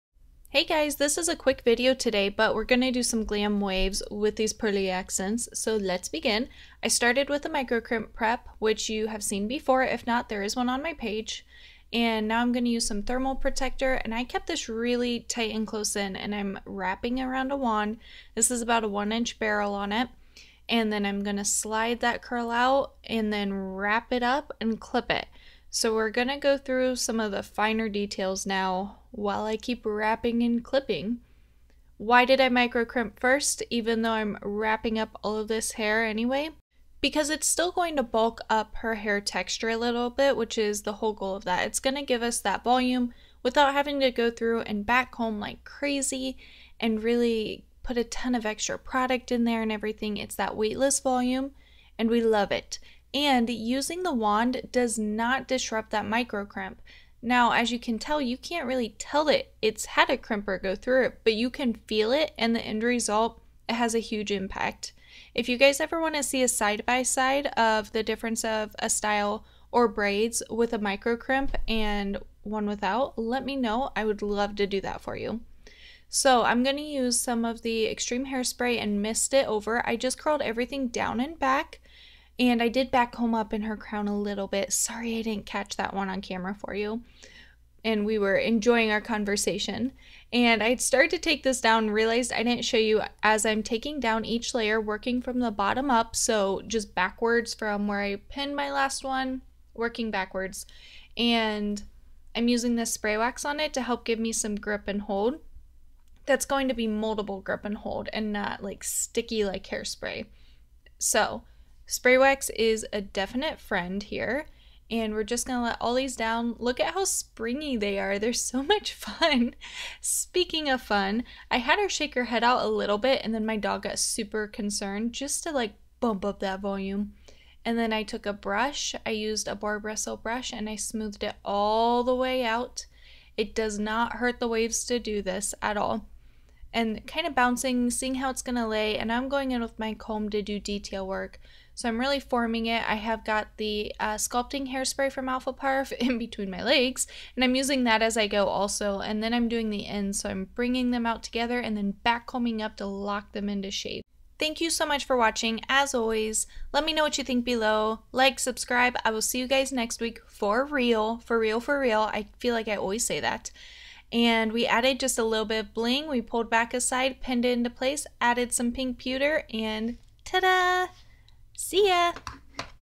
How to Create Glam Waves with Beauty by Haley Garber
SO SORRY I missed last week. I tried to upload too much at once and then left for a week and well, clearly, it didn't happen as planned. BUT. I have SO MUCH waiting for you guys, and we are going to be on it from here on out! So here we did some glam waves, it's a quick video but it's a classic. Biggest tips:
Use spray wax as you let them come down, and don't let them come down until they cool completely!

Products:
Alfaparf Milano thermal spray, spray wax, extreme hairspray and sculpting spray.
Lalas Tools Prep Tool for microcrimp
Paul Mitchell Unclipped Wand